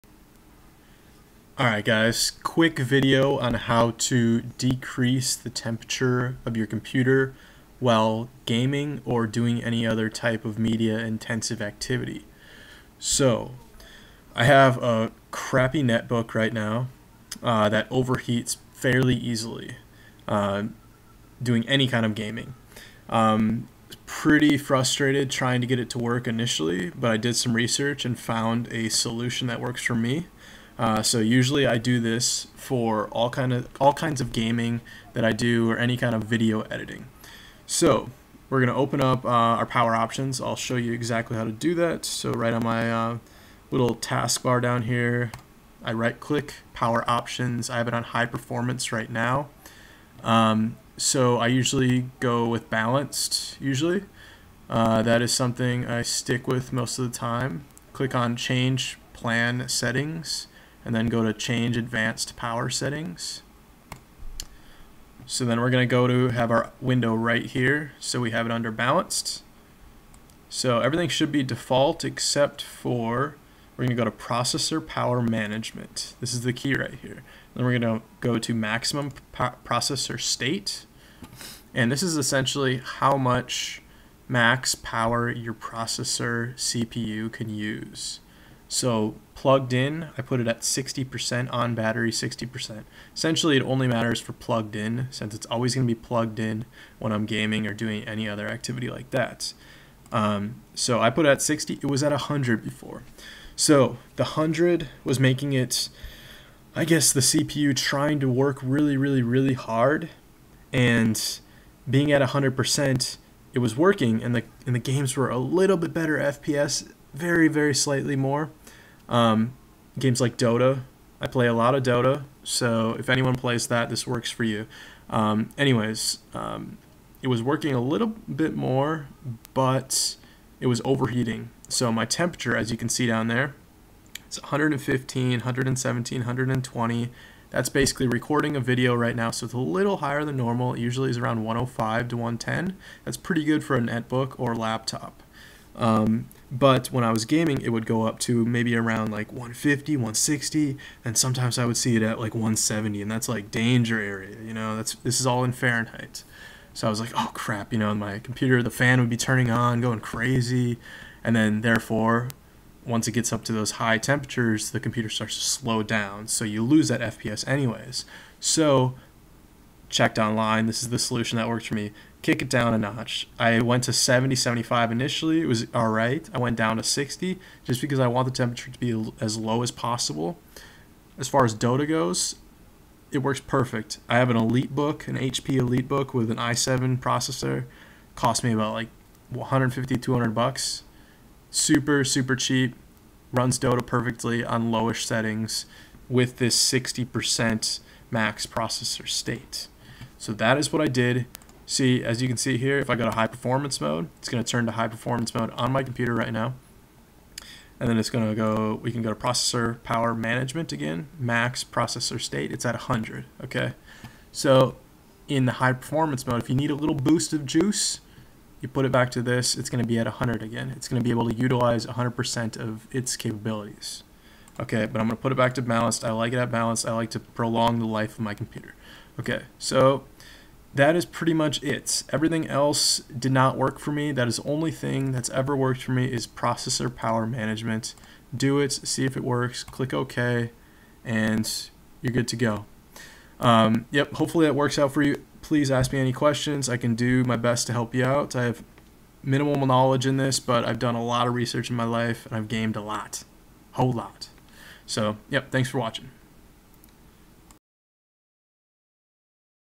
Stop Laptop from Overheating while Gaming [EASY SOLUTION]
EASY SOLUTION/FIX to decrease the temperature of your laptop, netbook, or desktop computer while gaming (Dota 2) or video/picture editing!

~STEPS~
Power Options - Change plan settings - Change advanced plan settings - Processor power management - Maximum processor state - Change it to a lower % - GAME ON!

~My laptop~
HP Elitebook 2560p
i7
8gb ram
windows 7

Adventure On!
On Wandering Facebook Page - "Like It" my friends!